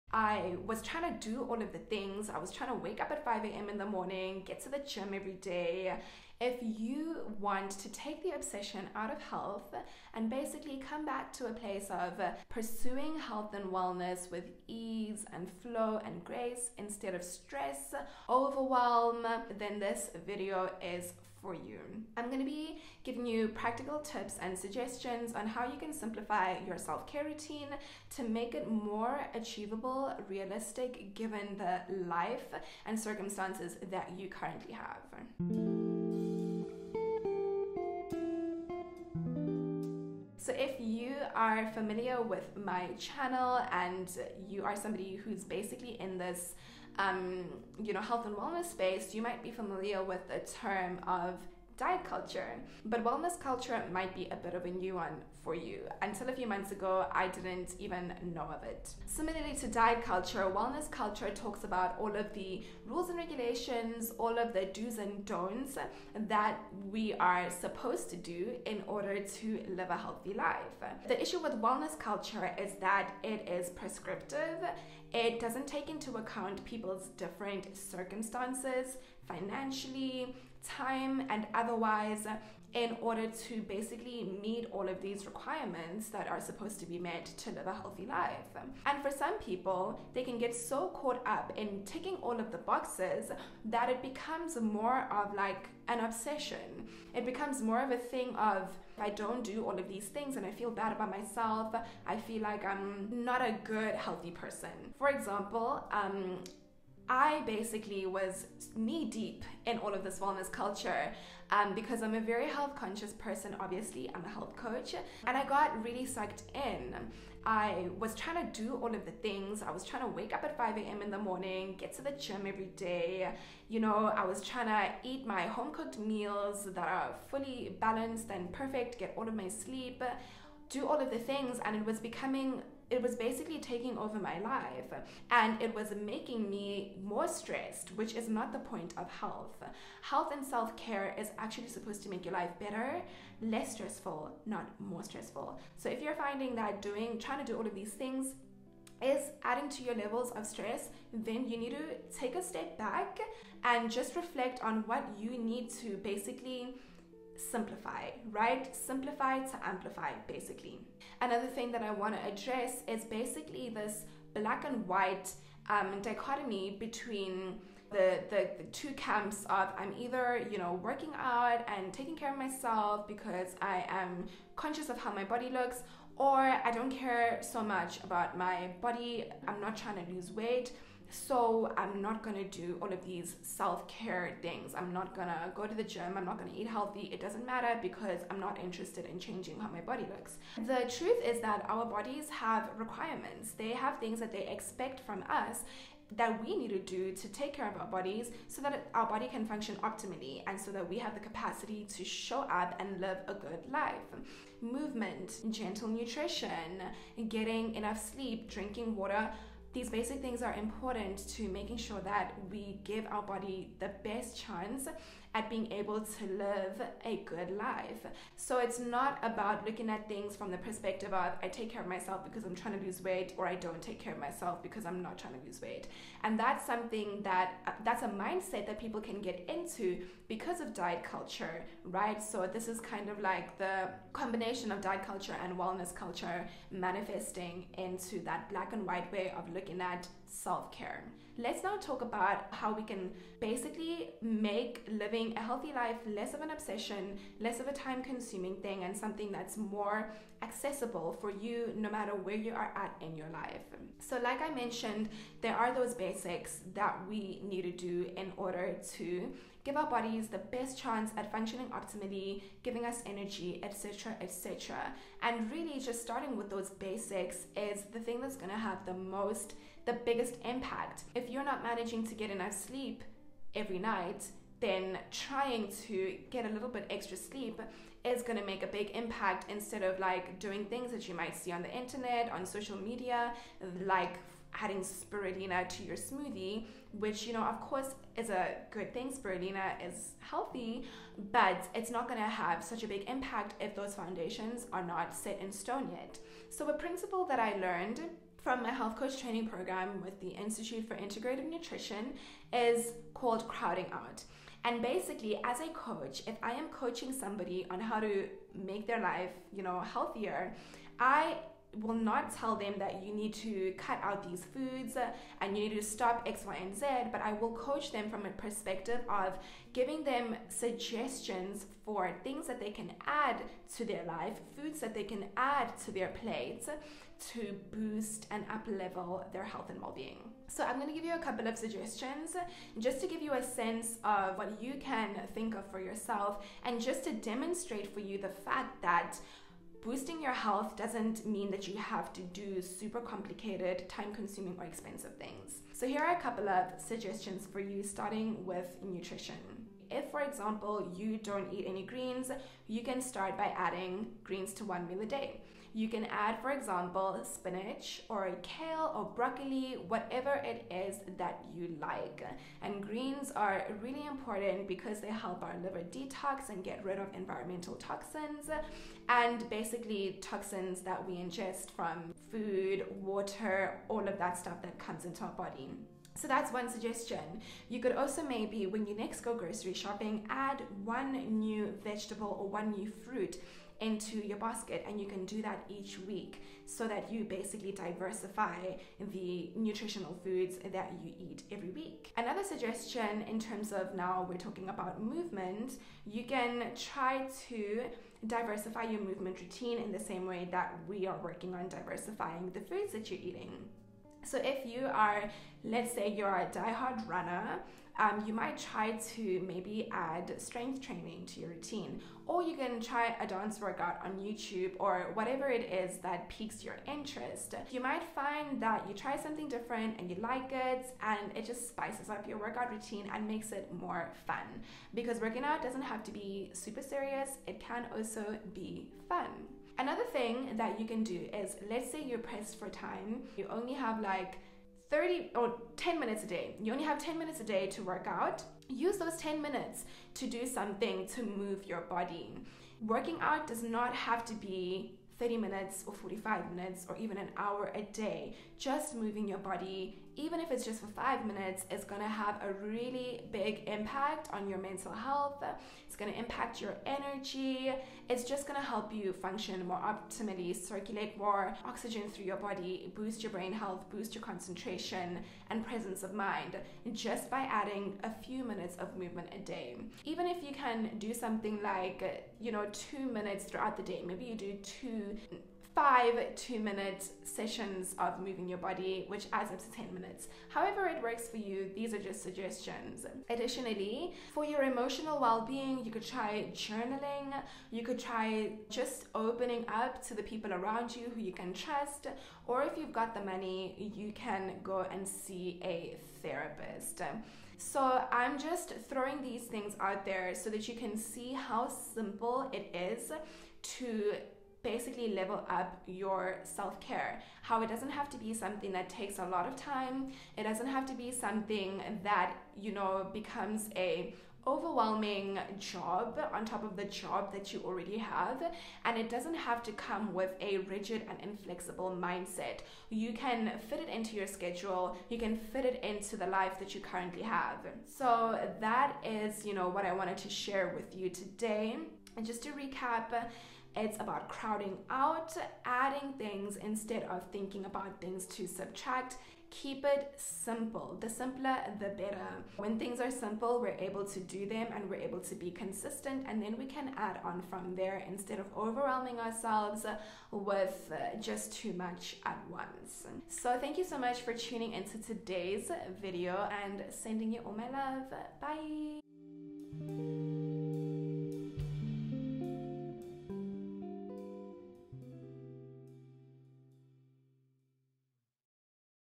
Realistic Self Care (Healthy Habits You Can Stick To)
Can't stay consistent with your healthy habits? 😞 I've learned over time that small and simple steps lead to long-term consistency and big-time benefits for your overall health. Watch for suggestions on how to up-level your self-care routine *without* adding a million things to your plate (that you won't be able to keep up anyway).